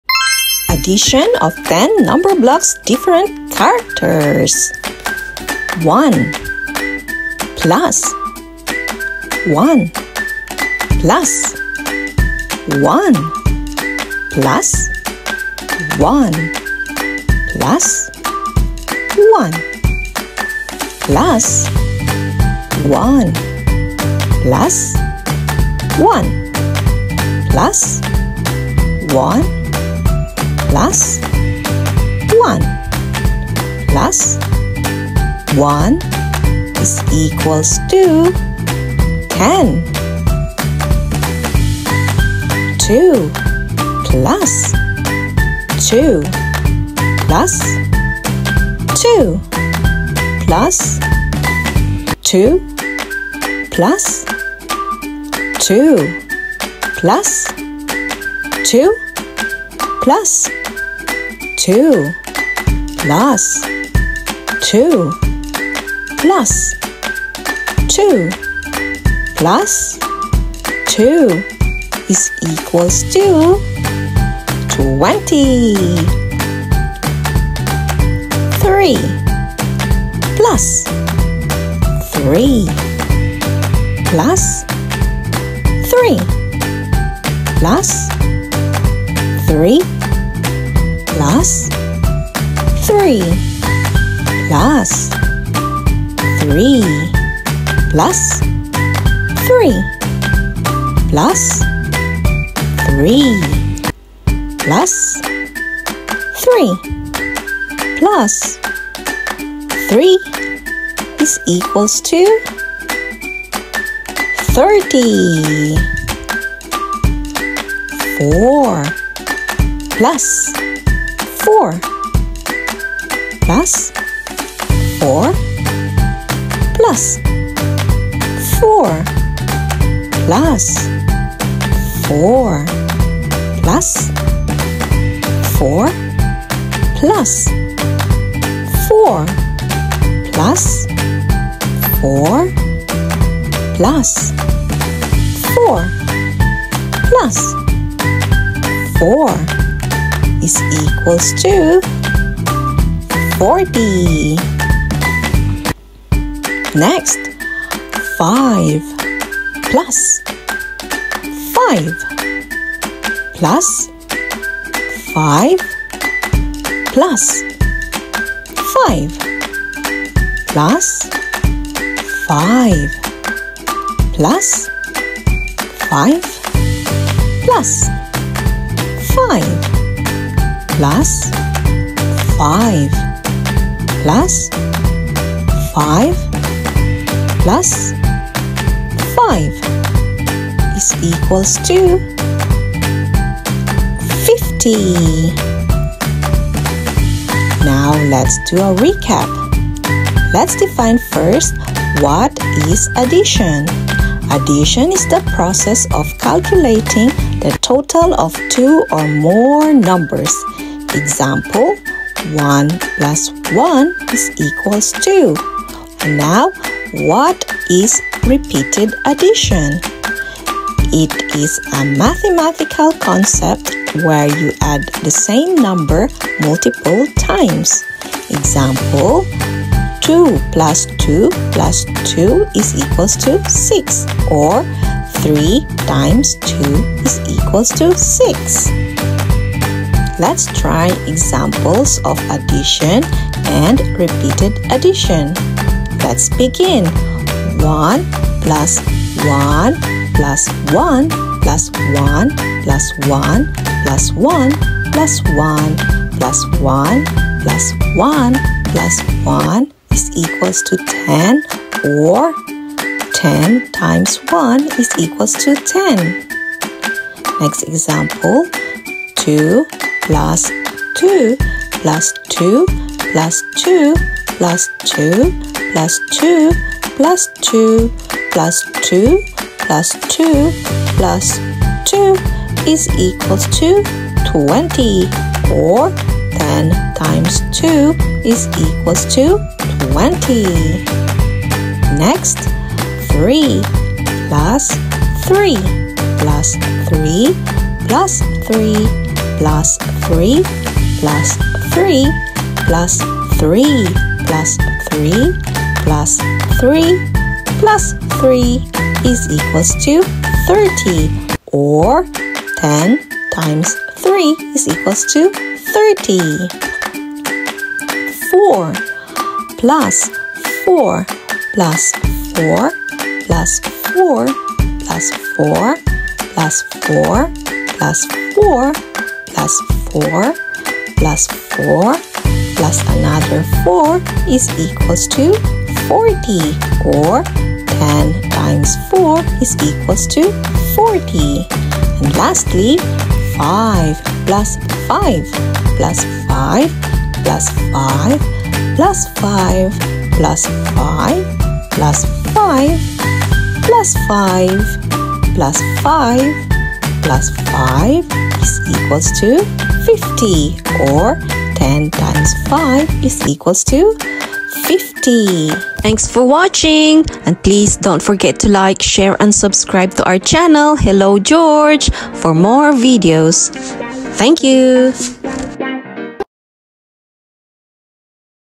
NUMBERBLOCKS ADDITION OF 10 DIFFERENT CHARACTERS | REPEATED ADDITION | LEARN TO COUNT | hello george
NUMBERBLOCKS ADDITION OF 10 DIFFERENT CHARACTERS | REPEATED ADDITION | LEARN TO COUNT
Hi and hello everyone! welcome to all numberblocks friends and subscribers! Today, let's learn about addition and repeated addition using 10 different numberblocks characters. let's define first what is addition! It is the process of calculating the total of two or more numbers, whole repeated addition is a mathematical concept where you add the same number multiple times. Always remember, learning maths is easy and fun with numberblocks!